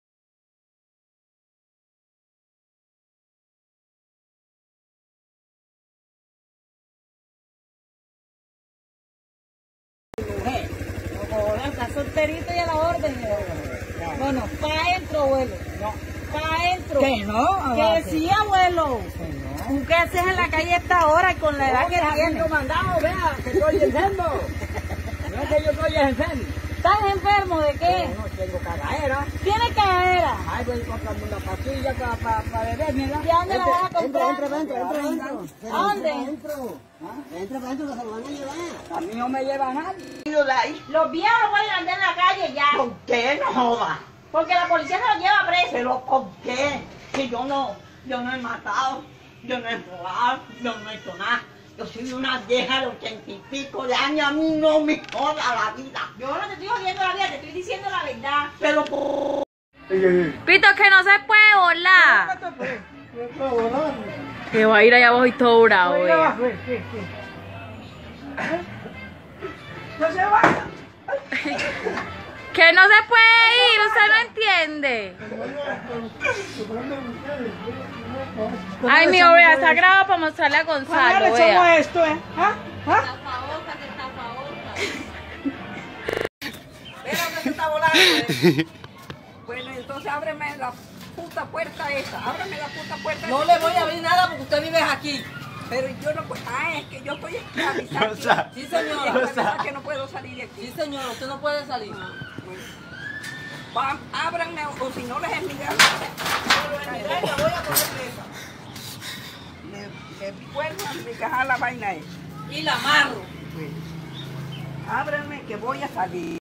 abuelos en cuerentena
nuestros abuelitos todos tercos, no se quieren quedar en casa, Dios los proteja y los cuide.